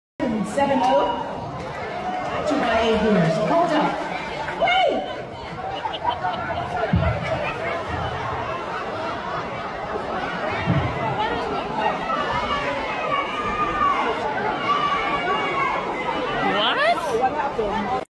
BombThreat Forces Evacuation Of PattiLaBelle Concert
A bomb threat forced the evacuation of a Patti LaBelle concert at a theater in Milwaukee, Wisconsin, organizers said.